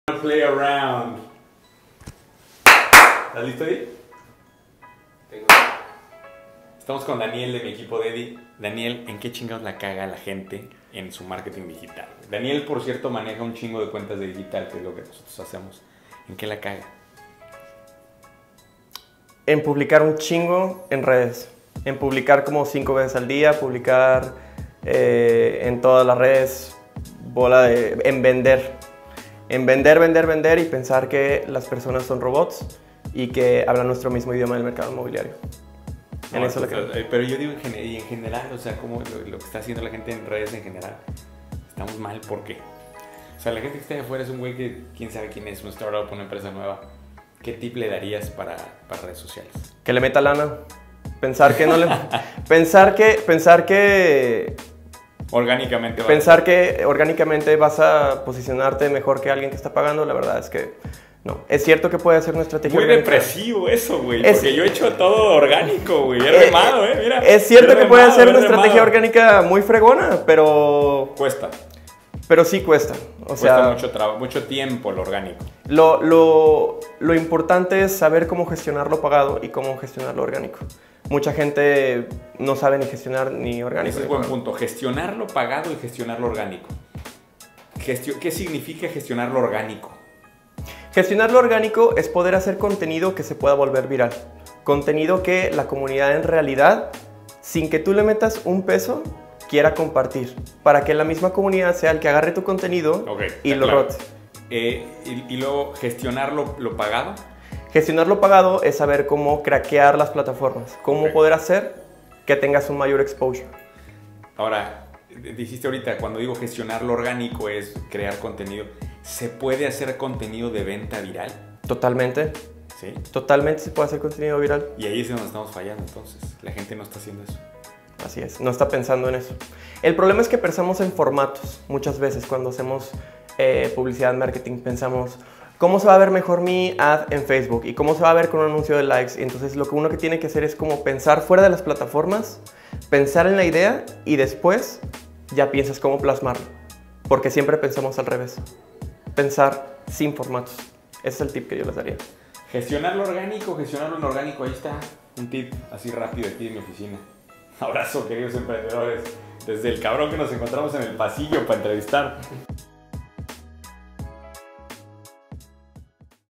¿Cómo ser viral? // Carlos Muñoz
Daniel maneja al rededor de 50 cuentas digitales. Hoy nos dice en qué la estamos cagando.